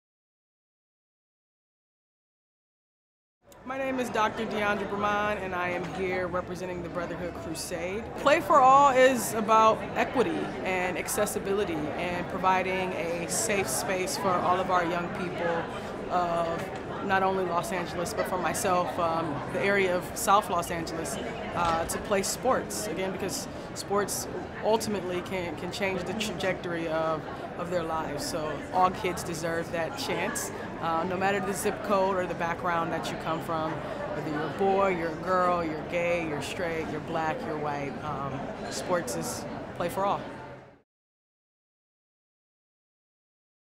Dr. Diandra Bremond Joins the PlayForAll Movement
USC Adjunct Lecturer Dr. Diandra Bremond spoke about the PlayForAll Movement and youth sports as a whole at the 2017 LA84 Foundation Summit on October 27, 2017 at the JW Marriott Los Angeles L.A. Live